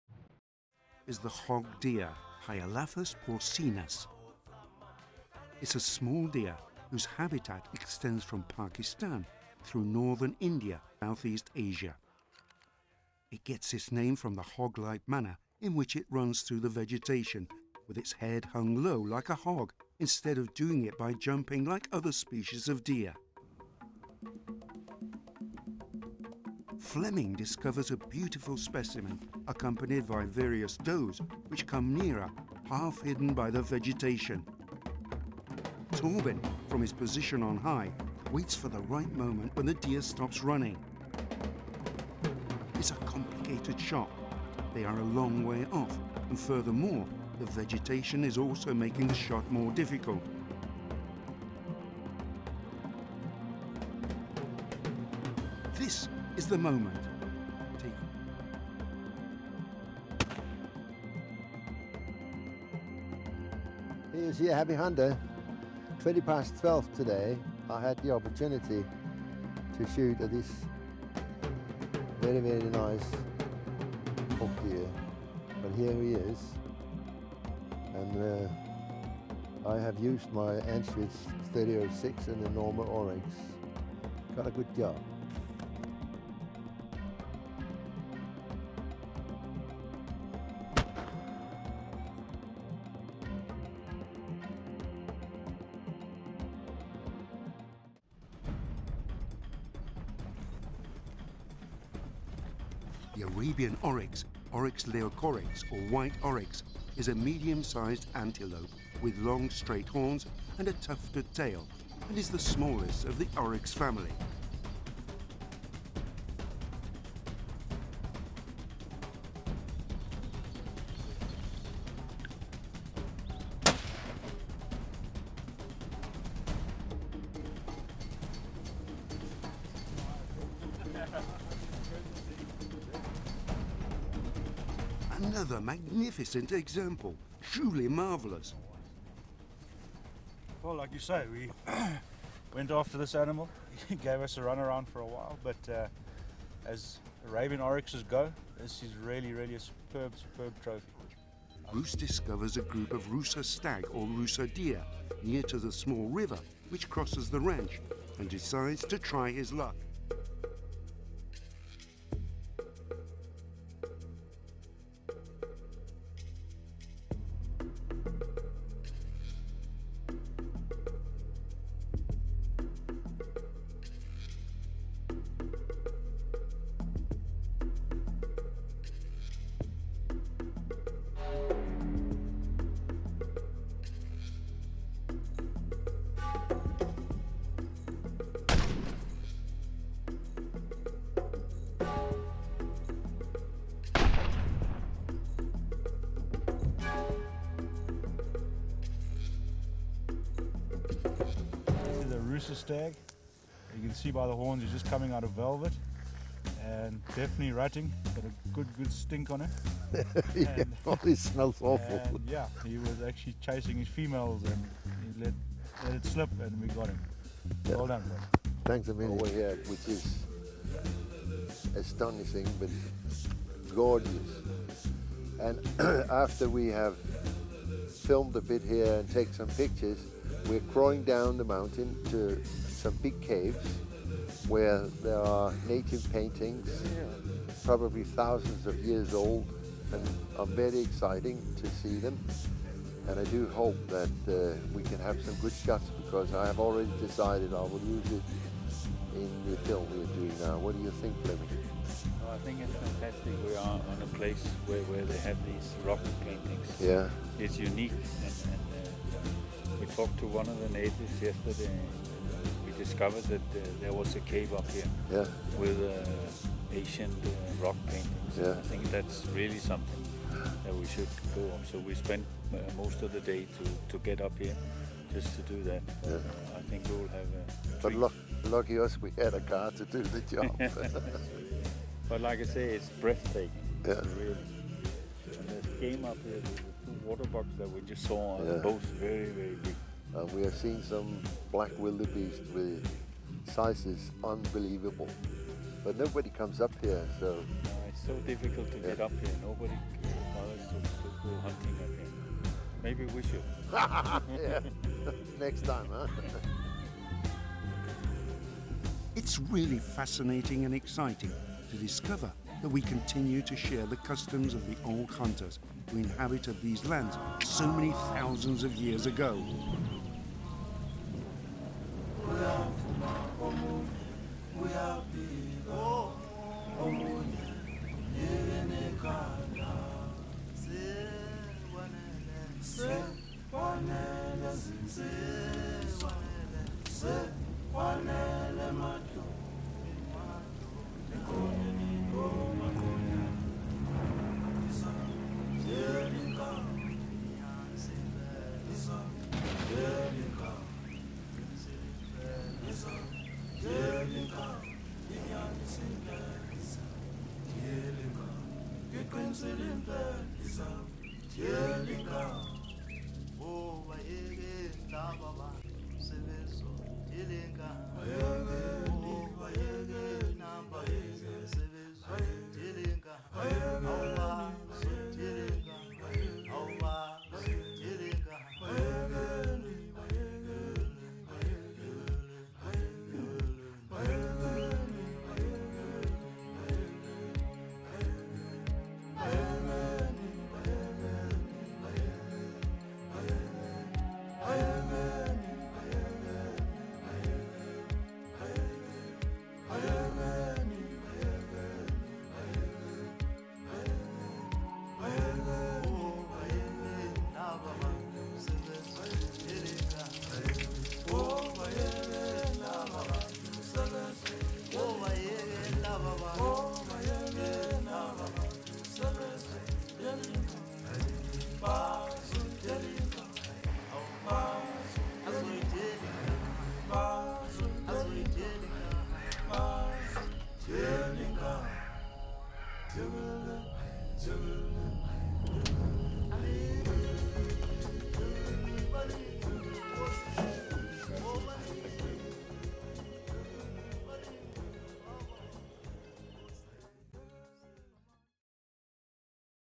Hogg Deer, Rusa Stag & Arabian Oryx Hunting
Hogg Deer, Rusa Stag & Arabian Oryx Hunting in South Africa with Side by Side Safaris